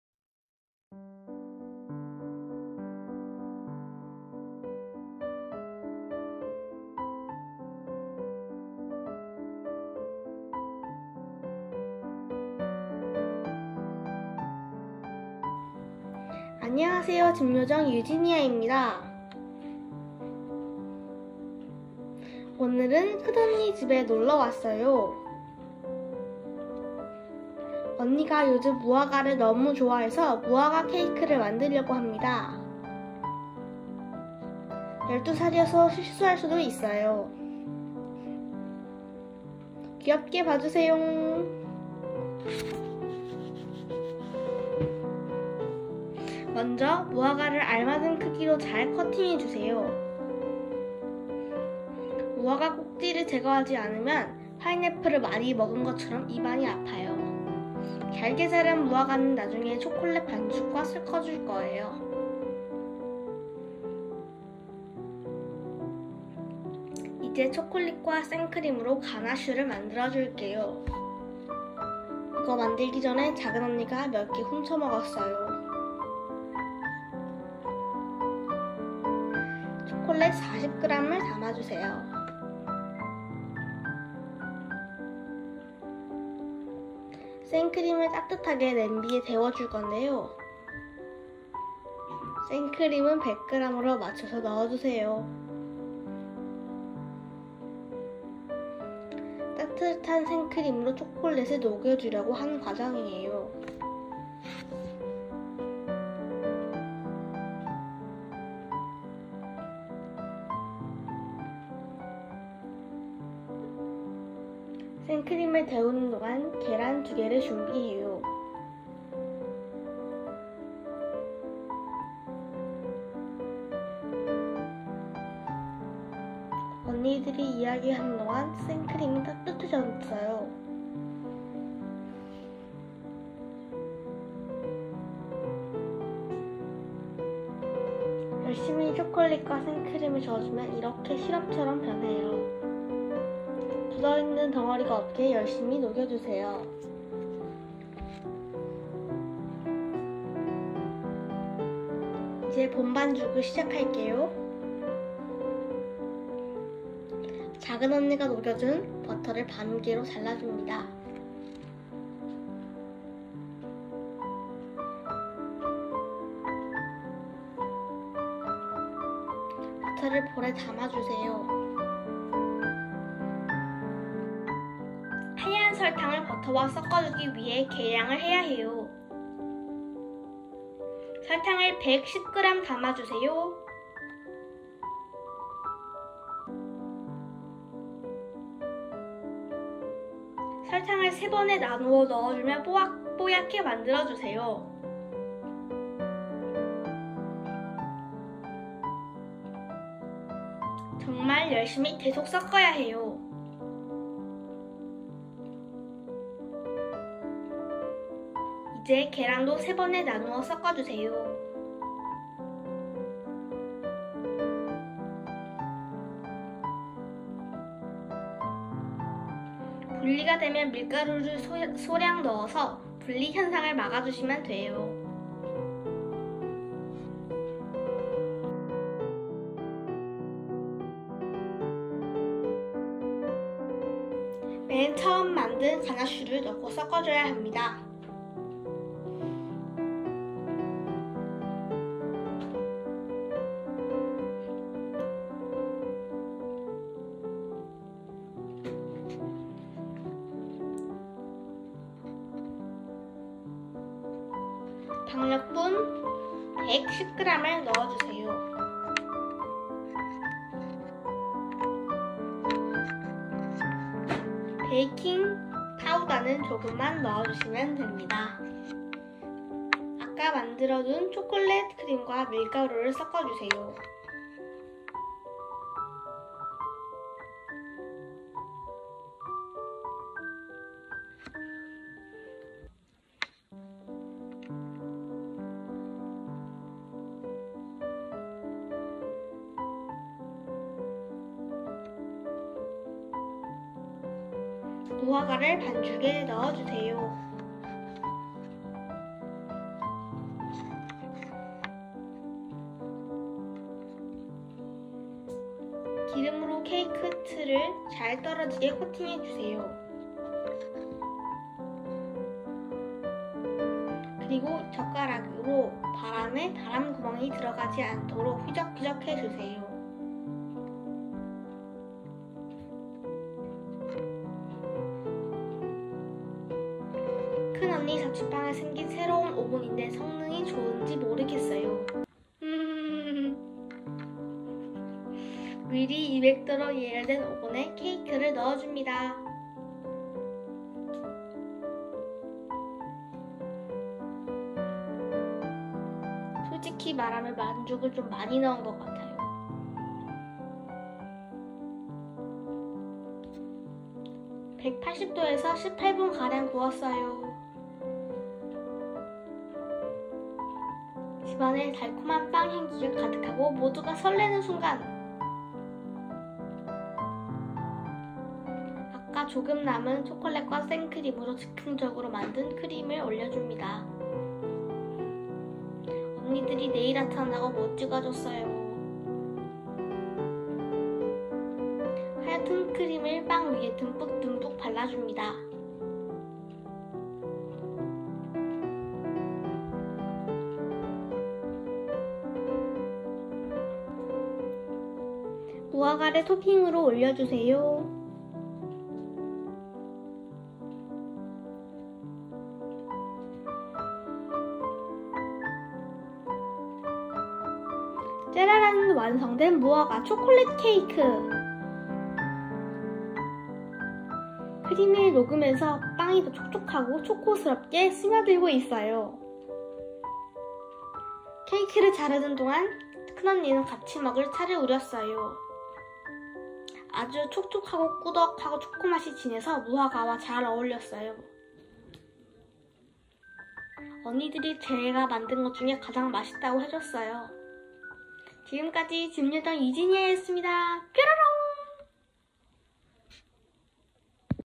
금손 13살이 언니 자취방에서 직접 만드는 홈베이킹 vlog 08년생/초6/무화과 초콜렛 케이크 fig chocolate cake 🍰
안녕하세요 루시 입니다!! 오늘은 막둥이 동생이 집에 놀러와서 무화과 초콜렛 케이크를 홈베이킹 해줬어요! 쪼끄미 손으로 너무나 맛있는 케이크!! "집요정 유지니아"의 홈베이킹 앞으로 더 기대해주세요!!

Hi guy! This is Lucy :) Today my baby sister came over to my house and baked me a fig chocolate cake!! It was so so good!! I hope you guys like "House Elf Eugenia"!!